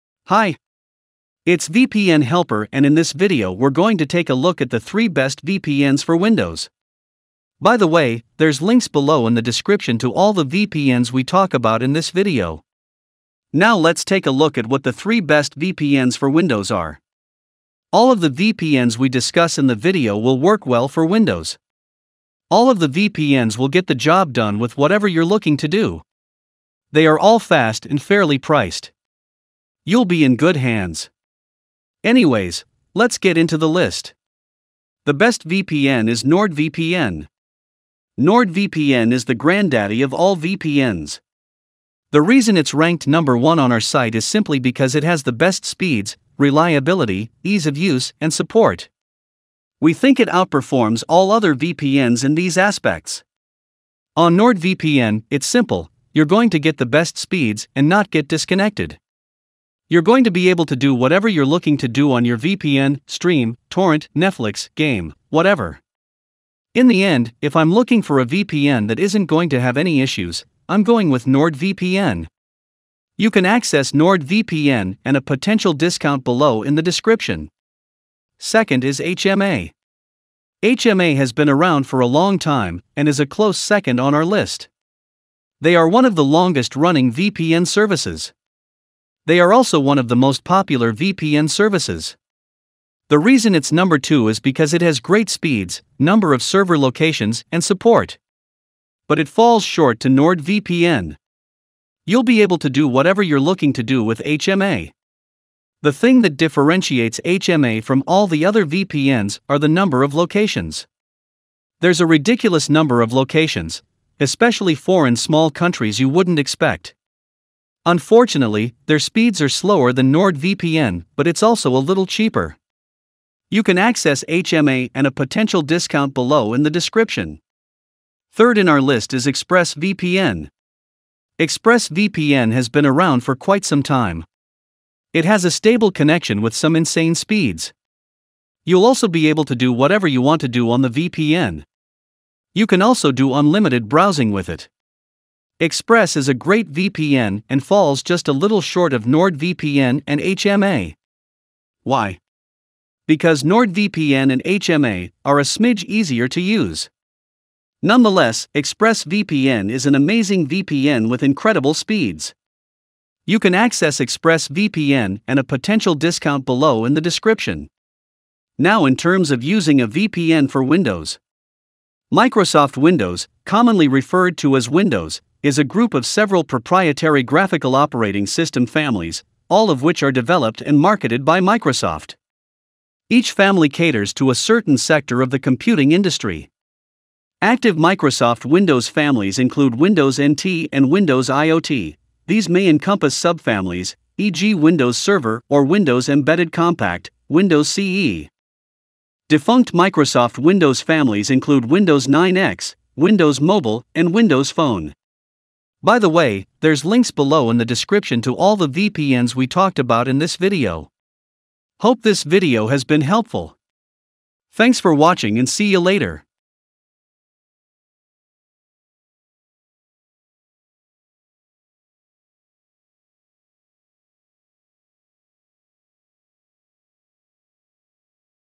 Best VPN for Windows Review 2025 – Fast, Secure & Windows-Friendly
- Read the full article here to find the best VPN for your needs.

Stay safe and enjoy fast, secure browsing on your Windows computer with VPNs that deliver top-tier speed, strong encryption, and seamless compatibility—perfect for streaming, gaming, and privacy.